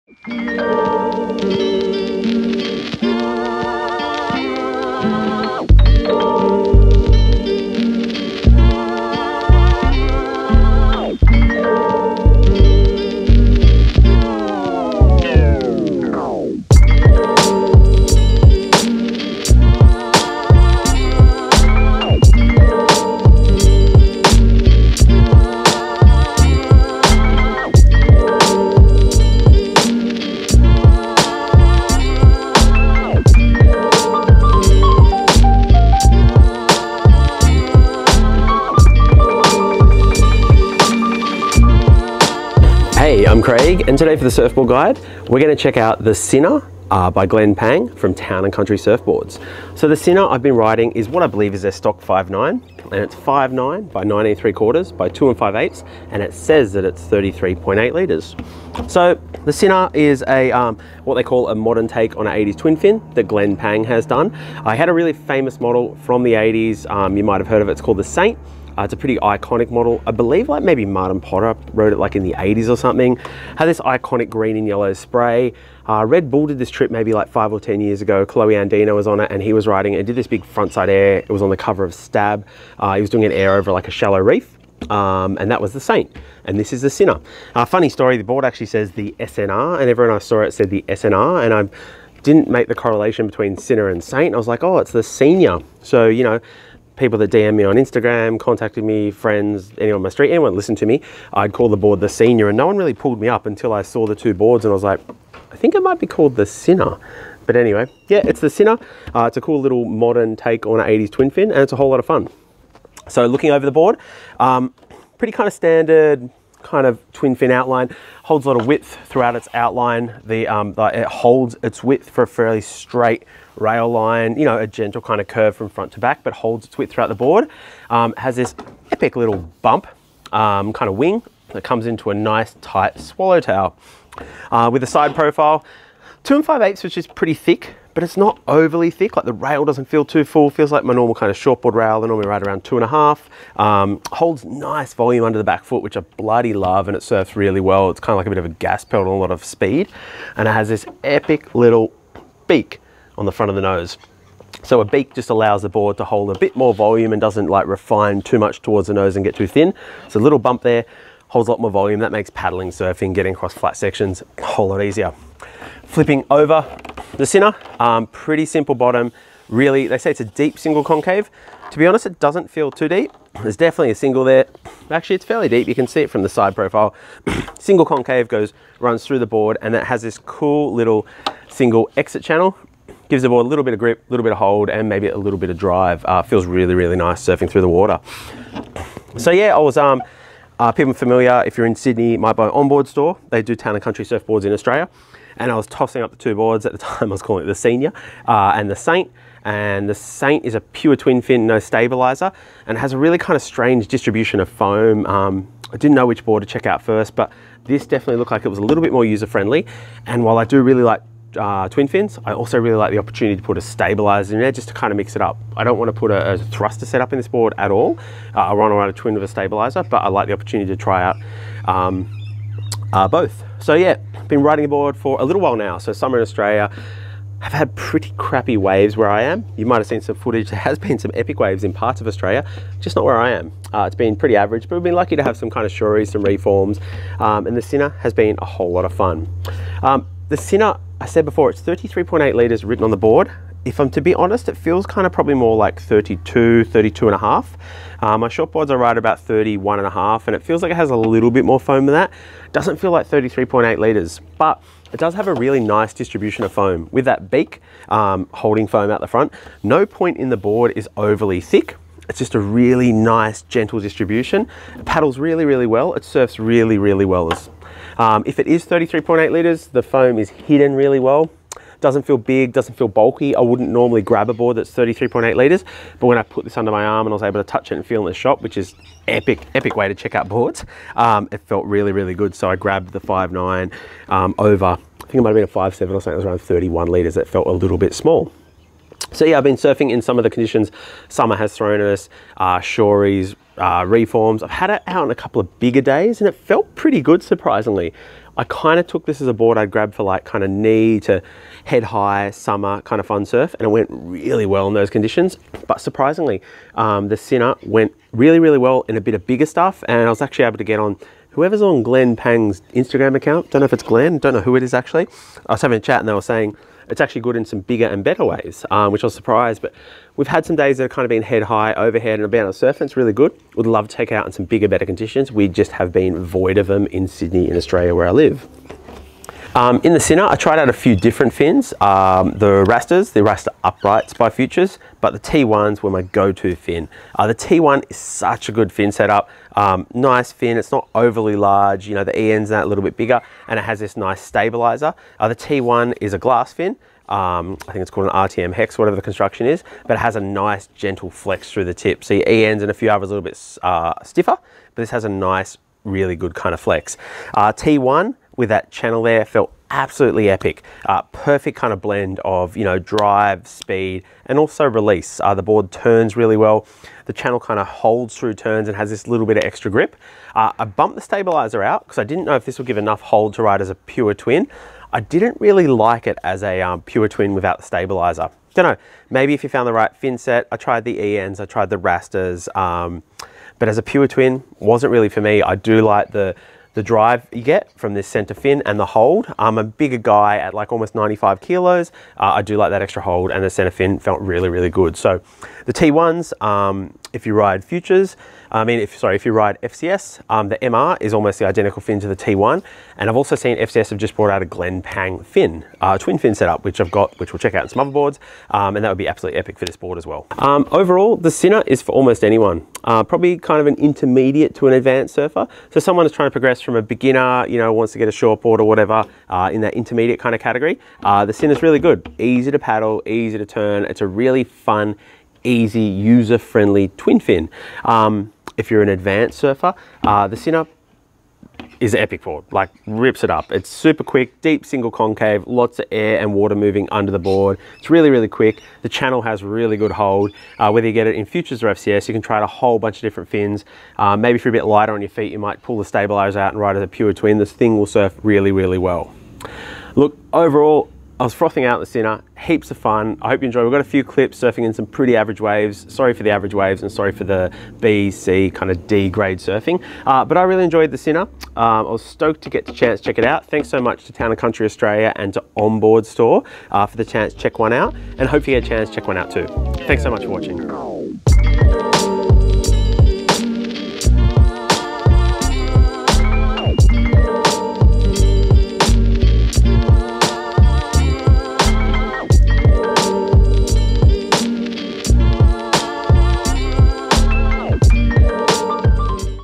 Town & Country (Glenn Pang) The Sinner + Futures T1 Twin Fin Review - The Surfboard Guide
Welcome to the review of the Town & Country "The Sinner" Twin Fin by Glenn Pang, which I have been riding in the stock 5'9 x 19 3/4 x 2 5/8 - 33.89L

People will likely be familiar with "The Saint" by Glenn Pang which was made famous by Martin "Pottz" Potter in the 80's. Well, the SNR (Sinner) is Glenn Pang's take on a modern twin fin compared to the Saint which is based off of the original, 80's design.

Through the bottom, The SNR carries a deep single concave, giving the board extra lift and speed through flatter, mushier sections. Through the tail, a wide single channel gives allot of traction and bite through the turns when you run the board as a straight twinny. The single wing and swallow tail, adds to the board's performance.

In the Sinner I rode a few different twin fin setups, but I felt that the T1 by Futures worked best with it.

The T1 Honeycomb Twin +1 is Future's twin-fin design for down the line drive, speed and smooth rail-to-rail feel that modern twin-fin surfing is known for. Made with honeycomb construction, the medium flex pattern offers a softer feel than fins from solid fiberglass, helping to create additional lift and drive in softer conditions.
The set is equipped with the option to ride with a small trailer fin for increased stability, creating more of a thruster feel.

Template Category | Neutral (balanced, all-around)
Construction | Honeycomb
Size | Medium Twin +1

Thanks for checking out the review of The Sinner by Town & Country Surfboards, and hopefully you get a chance to check one out!

Be sure to check us out @thesurfboardguide on Instagram to be across all the boards we are checking out, and to let us know what boards you want to see reviewed!